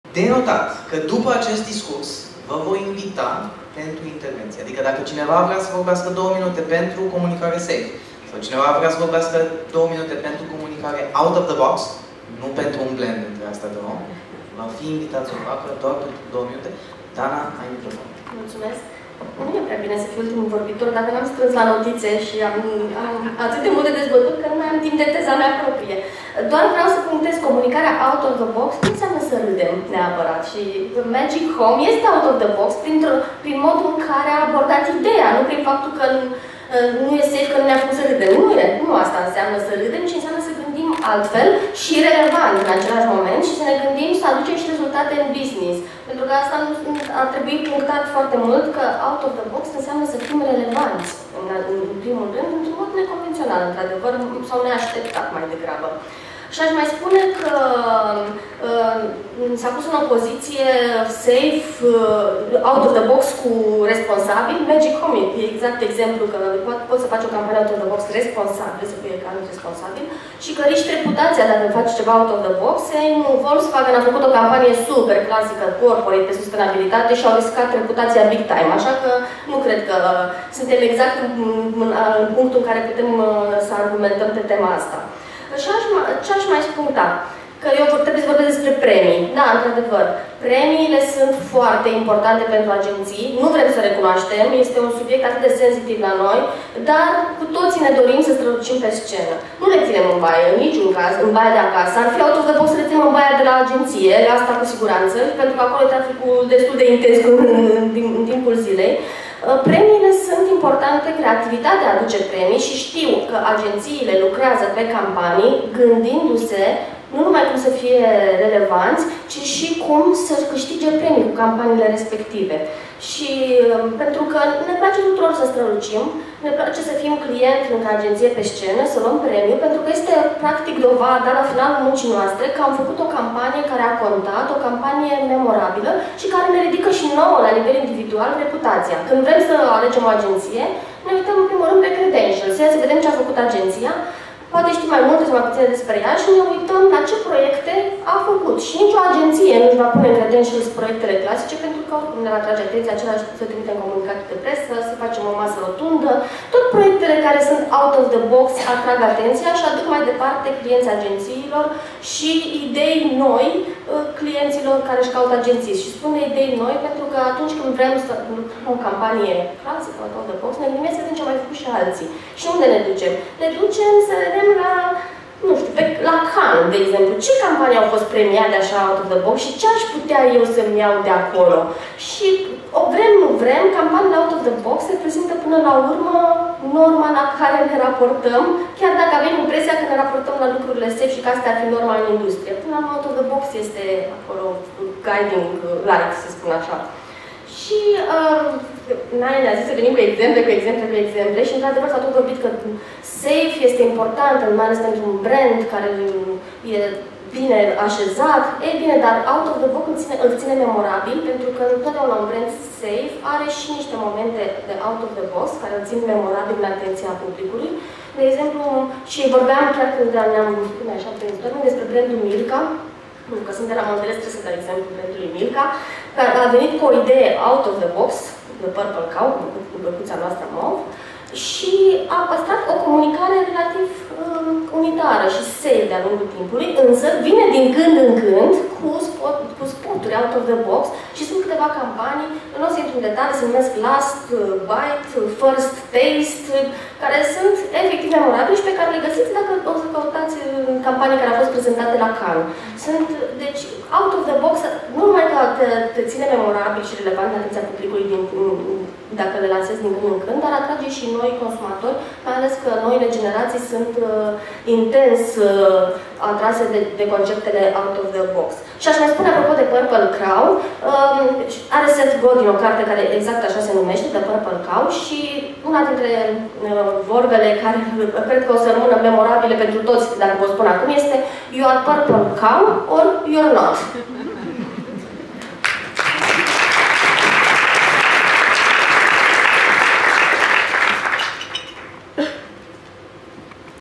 (15) Dana Dobrescu - Discurs Comunicarea Safe vs. Out of the box (2018.10.19, Hotel Radisson)
Aflată la cea de-a opta ediţie, Biz PR Awards 2018, conferinţa dedicată PR-ului şi comunicării digitale în era web-ului social, a prezentat cele mai noi instrumente de lucru şi tehnologii pe care le au la dispoziţie profesioniştii din domeniu.

Am aflat alături de cei mai buni specialişti din domeniu tendinţele care modelează PR-ul viitorului şi reperele care dau tonul schimbării!

Conferința a avut loc la hotel Radisson din București, în data de 19 octombrie 2018.